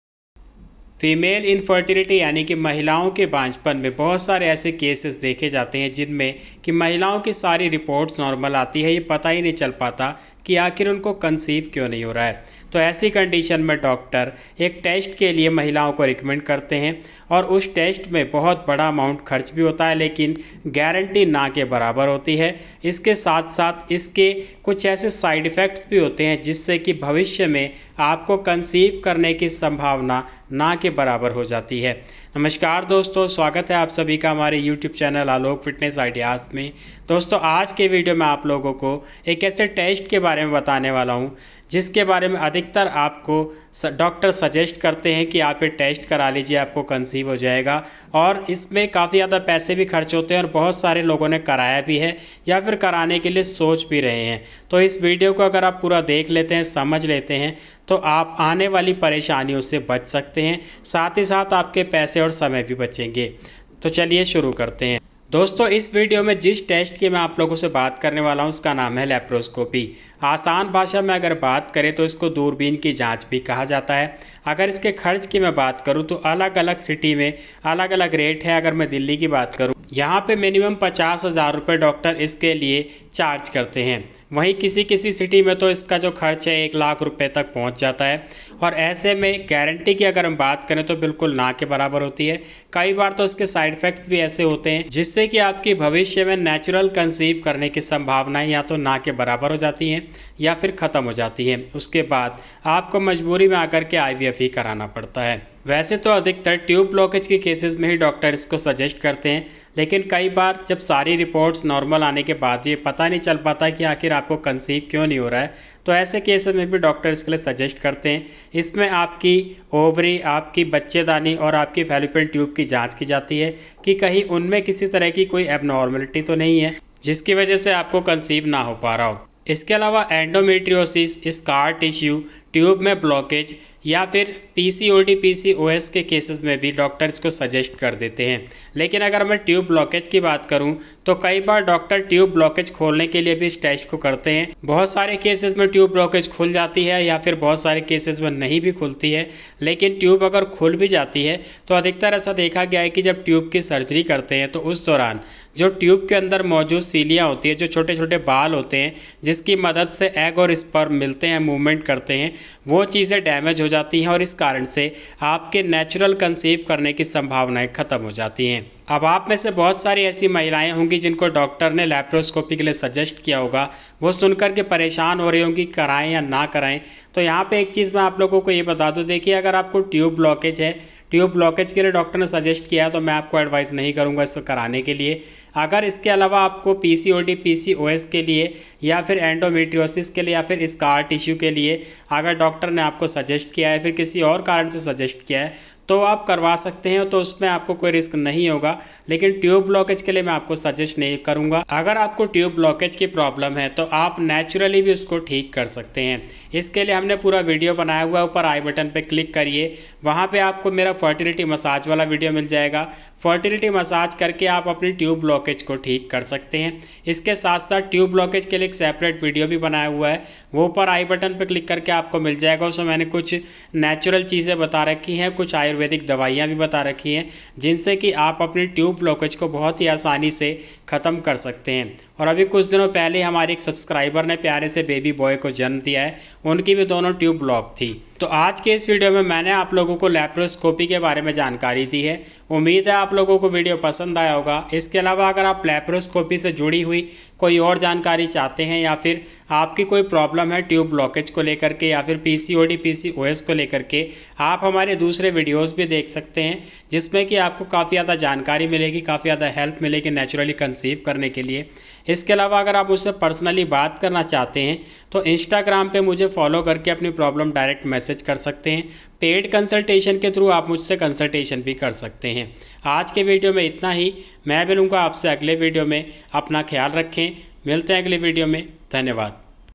मां बनना चाहती हैं तो भूलकर भी ये टेस्ट न कराएं डॉक्टर आपको बार बार कहेंगे
माँ बनना चाहती हैं तो भूलकर भी ये टेस्ट ना करायें डॉक्टर बार बार कहेंगे